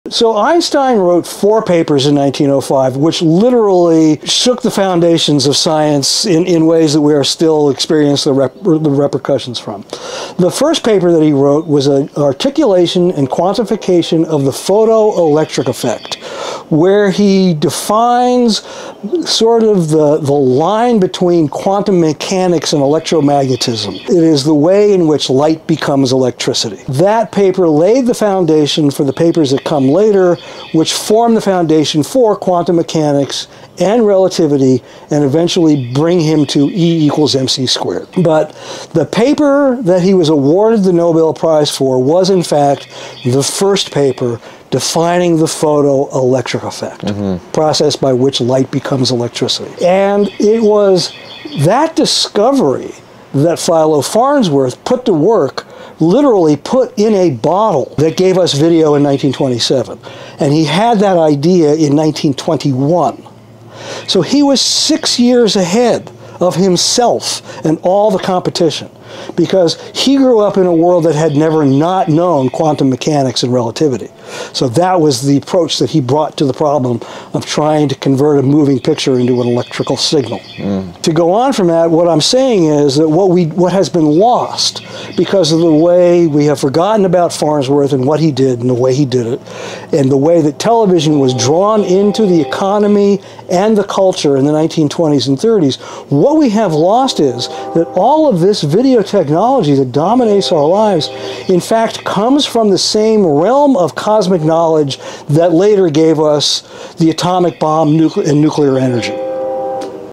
Your TV Comes From Nuclear Physics
Paul Schatzkin @driver49  drops cosmic truth:Einstein's 1905 paper on photoelectric effect = Nobel Prize"The way light becomes electricity."

This led to:

- Quantum mechanics
- Relativity
- E=mc²

But ALSO: Philo Farnsworth used it to invent television (1927).

"Same realm of cosmic knowledge that gave us atomic bomb and nuclear energy."

Your screen and nukes?

Same physics.
Same source.

We forgot the cosmic origin of our screens.